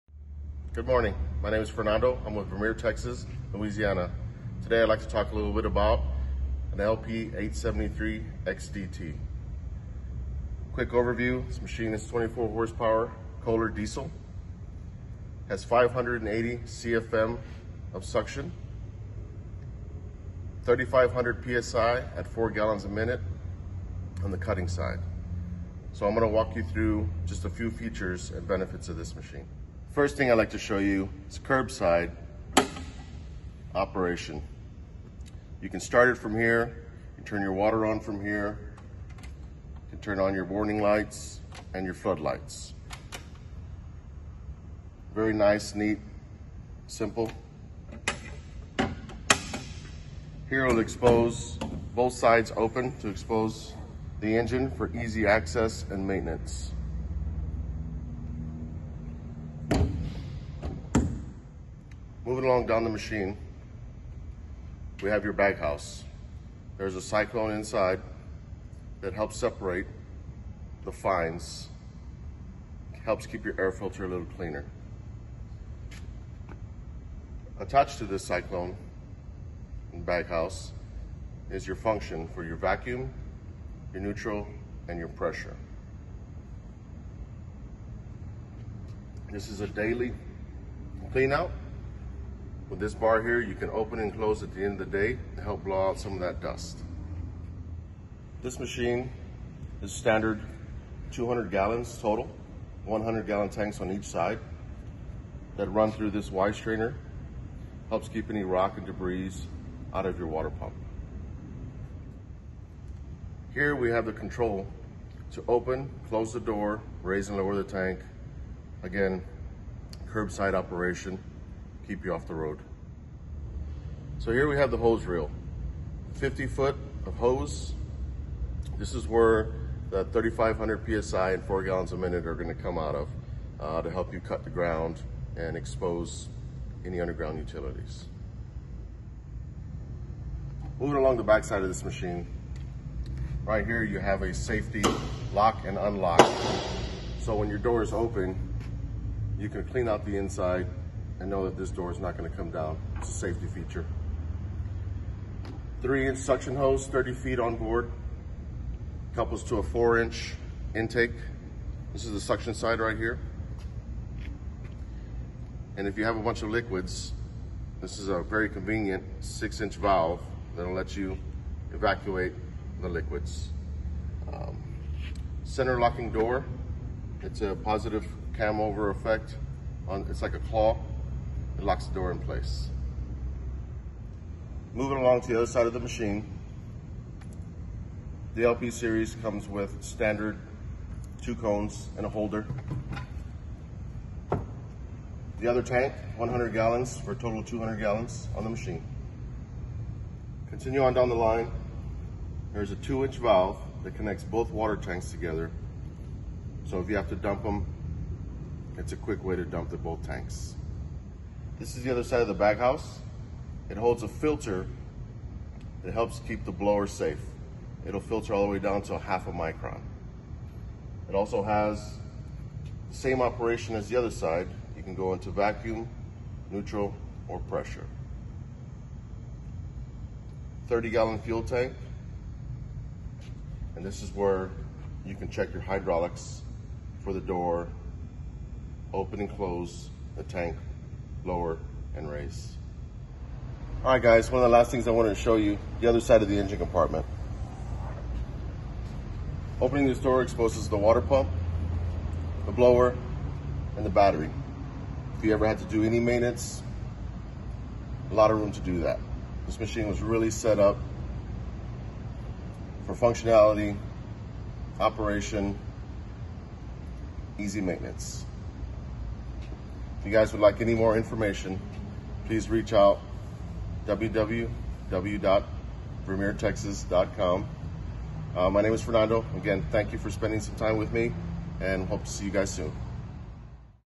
Vermeer LP 873 XDT Vacuum and Hydro Excavator
Join Vermeer Texas-Louisiana sales representative in Selma (San Antonio), Texas for a virtual walk-around of the Vermeer LP 873 XDT vacuum excavator. This equipment offers easy functionality, maintenance and operation for those working in the underground utility industry. Vermeer Texas-Louisiana ... Equipped to do more.